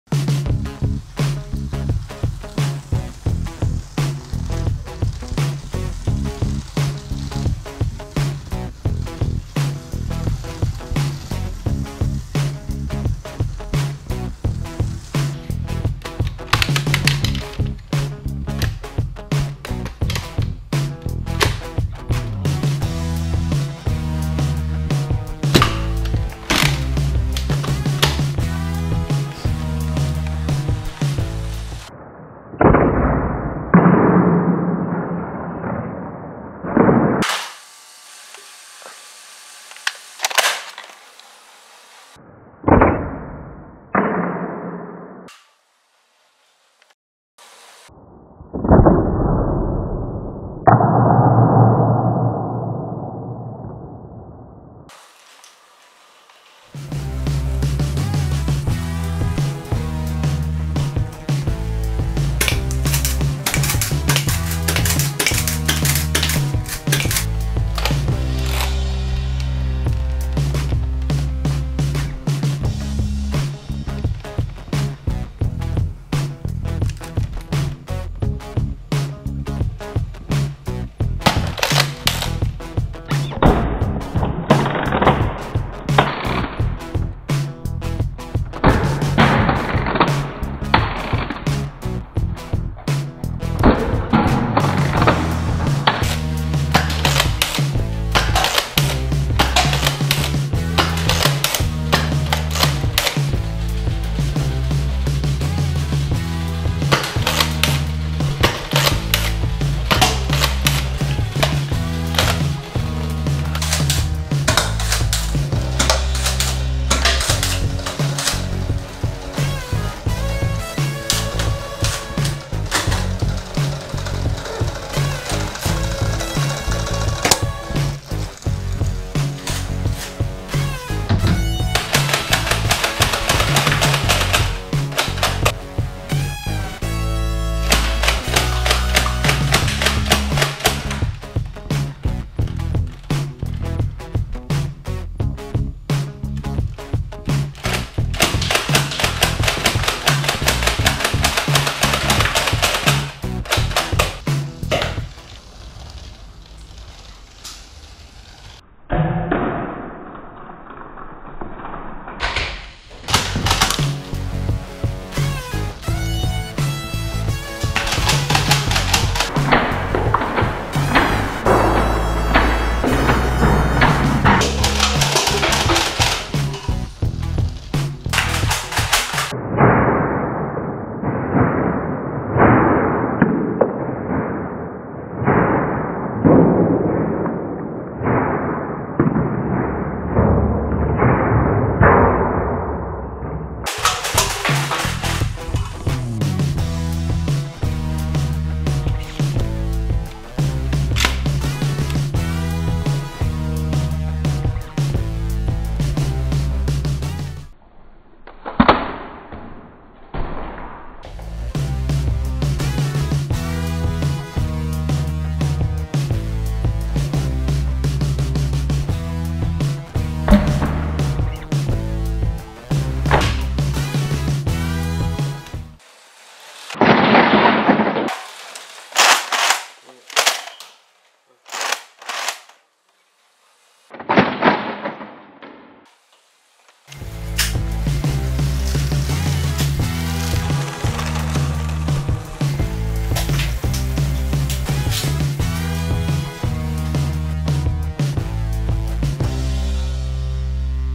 Lego City train with Nerf target
Lego train carries a target for Nerf gun shooting practice .
We used Lego City Passenger train and lego train tracks.  We used NERF Zombie Strike Sledgefire Blaster that is modified to shoot single darts, also NERF Zombie Strike Slingfire shotgun style  lever action, and Nerf Modulus Tri-Strike, also Nerf Missile Launcher